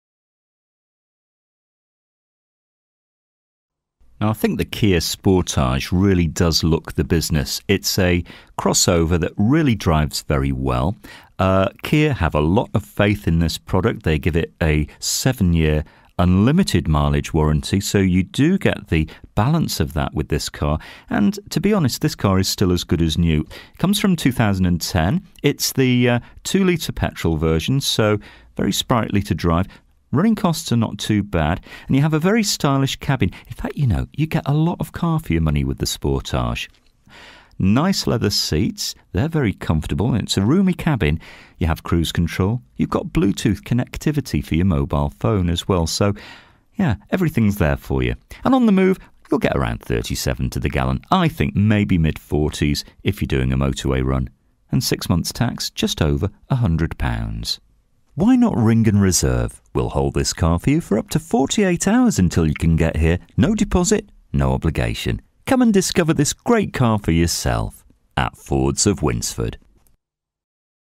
KIA SPORTAGE ESTATE SPECIAL EDITION (2010) 2.0 FIRST EDITION 5DR - YP60DZH
DETAILS:
First registered: 17/11/2010
Current mileage: 10260
Registration: YP60DZH
Colour: BLACK

This used car is for sale at Fords of Winsford Car Supermarket in Cheshire, UK